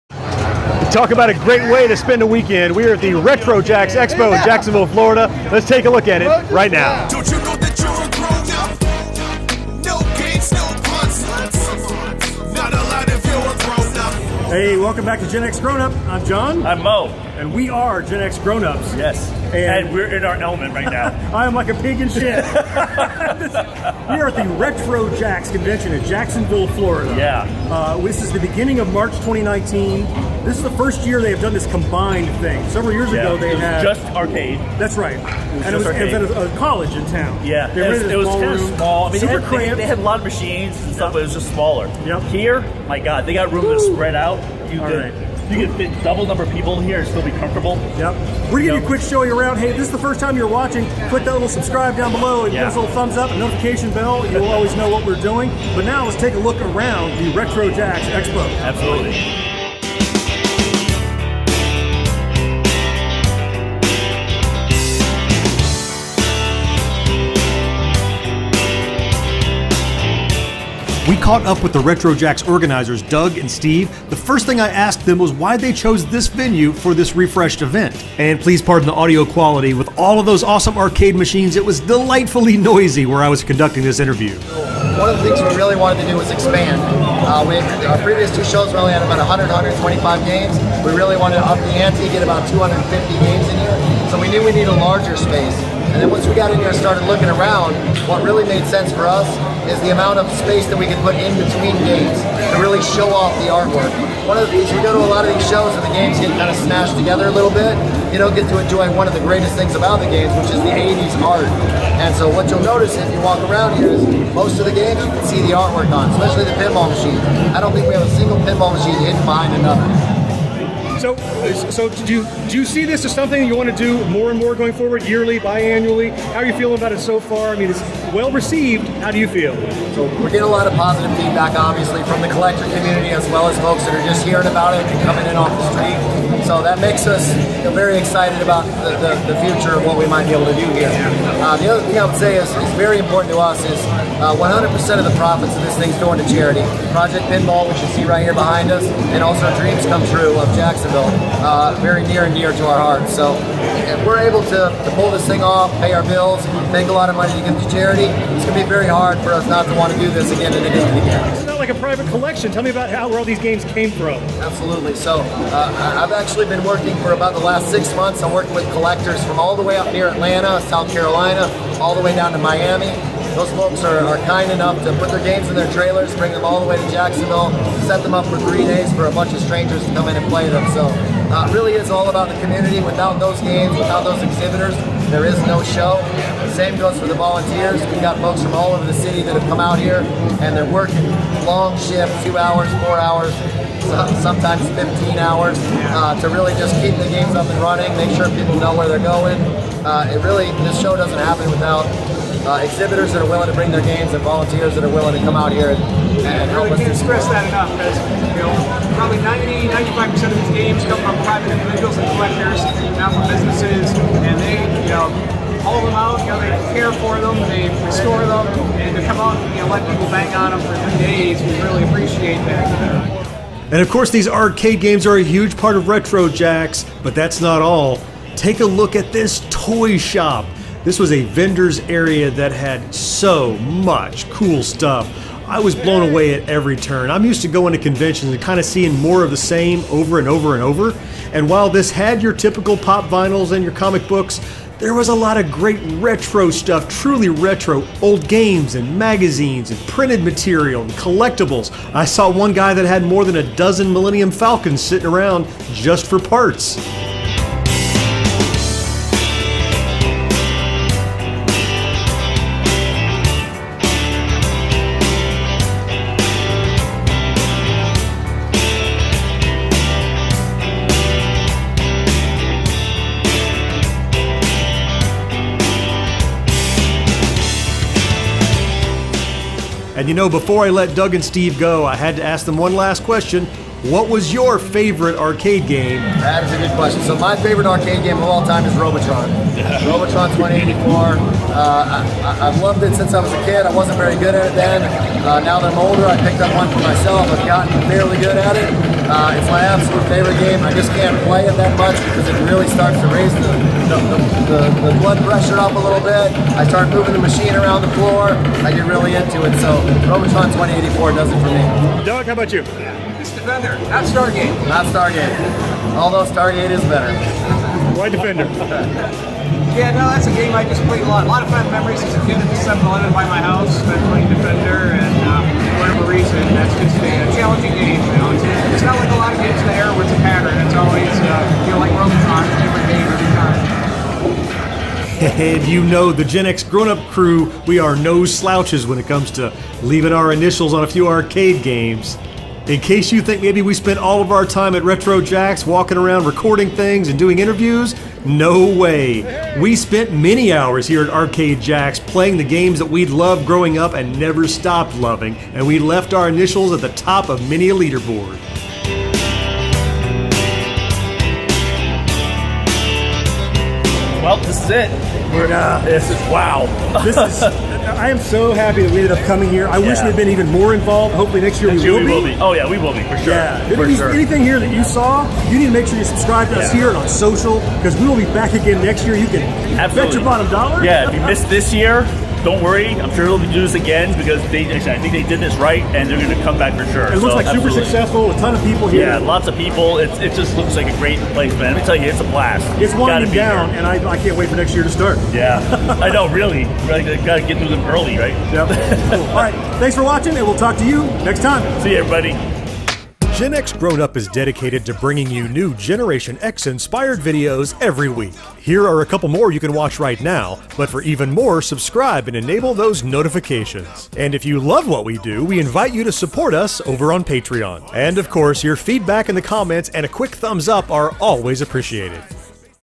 RetroJAX Weekend 2019 | Classic Arcade Games & Collectibles!
Jax Arcade & Pinball Expo and Retrorama Collectibles formed a strategic partnership to deliver Northeast Florida’s premier retro pop culture exposition, RetroJAX. This alliance is designed to bring fan favorites from both shows together through a blend of classic pinball and arcade gaming, vintage collectibles, and pop culture fun!

RetroJAX 2019 came to Jacksonville, Florida at the Morocco Shrine Auditorium the weekend of March 8-10, 2019. This venue boasts over 30,000 square feet of event space allowing the show to offer exhibitors, vendors, and fans an experience larger, richer and more diverse than ever before.